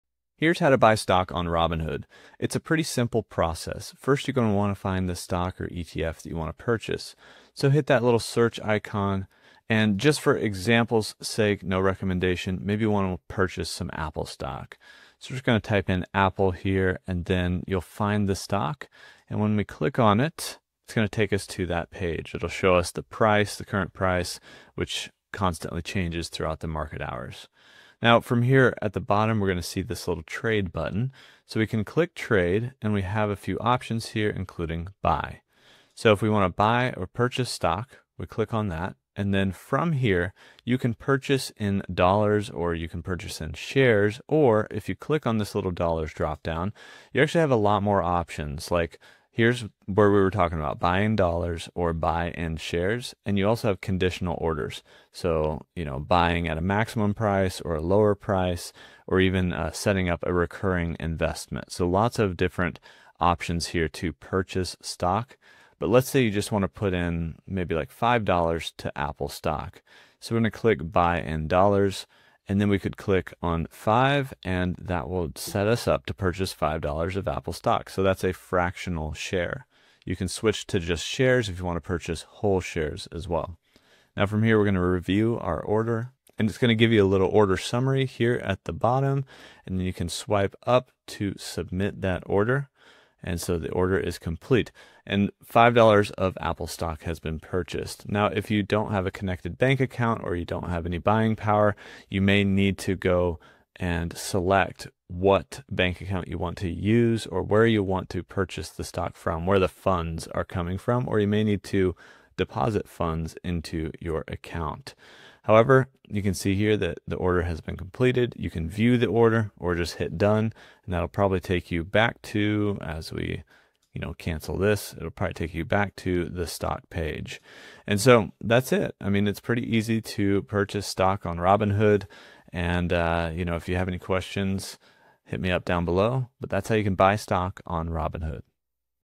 How to Buy Stock on Robinhood
In this Robinhood tutorial, learn how to buy stock on Robinhood. This guide explains how to place an order, choose between market or limit orders, and confirm your purchase in the app.

Get a free stock on Robinhood!